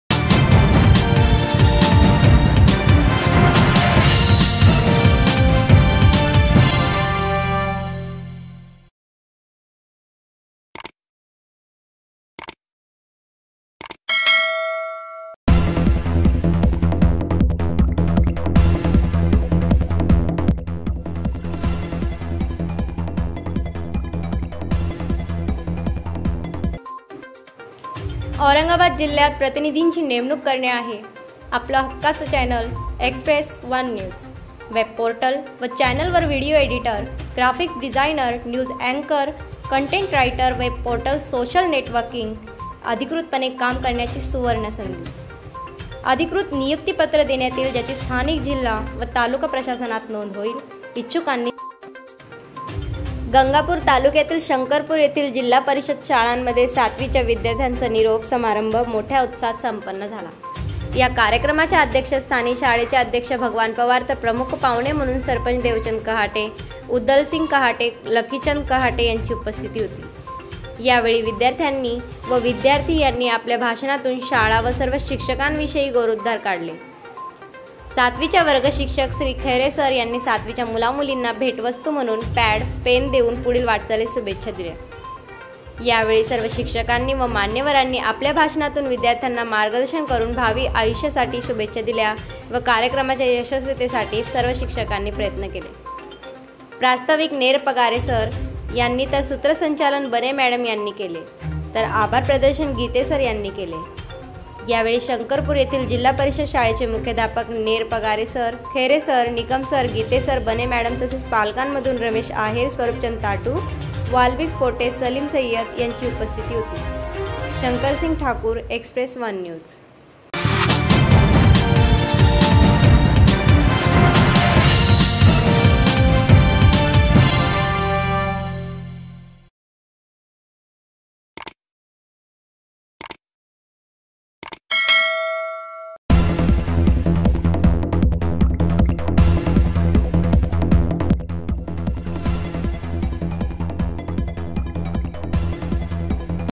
शंकरपुर येथील जिल्हा परिषद शाळांमध्ये सातवीच्या विद्यार्थ्यांचा निरोप समारंभ उत्साहात संपन्न झाला.
या कार्यक्रमाच्या अध्यक्षस्थानी शाळेचे अध्यक्ष भगवान पवार तर प्रमुख पाहुणे म्हणून सरपंच देवचंद कहाटे, उदलसिंग कहाटे, लखिचंद कहाटे यांची उपस्थिती होती. यावेळी विद्यार्थ्यांनी व विद्यार्थी यांनी आपल्या भाषणातून शाळा व सर्व शिक्षकांविषयी गौरवोद्गार काढले. यावेळी सातवीच्या वर्गशिक्षक श्री. खैरे सर यांनी सातवीच्या मुला मुलींना भेट वस्तू म्हणून पॅड, पेन देऊन पुढील वाटचालीस शुभेच्छा दिल्या. यावेळी सर्व शिक्षकांनी व मान्यवरांनी आपल्या भाषणातून विद्यार्थ्यांना मार्गदर्शन करून भावी आयुष्यासाठी शुभेच्छा दिल्या व या कार्यक्रमाच्या यशस्वीतेसाठी सर्व शिक्षकांनी प्रयत्न केले. यावेळी प्रस्ताविक नेरपगारे सर यांनी तर सुत्रसंचलन बने मॅडम यांनी केले तर आभार प्रदर्शन गिते सर यांनी केले. यावेळी शंकरपुर येथील जिल्हा परिषद शाळेचे मुख्याध्यापक नेरपगारे सर, खैरे सर, निकम सर, गिते सर, बने मॅडम तसेच पालकामधुन रमेश आहेर, स्वरुपचंद ताटु, वाल्मिक पोटे, सलीम सय्यद यांची उपस्थिती होती.

शंकरसिंग ठाकूर express१ news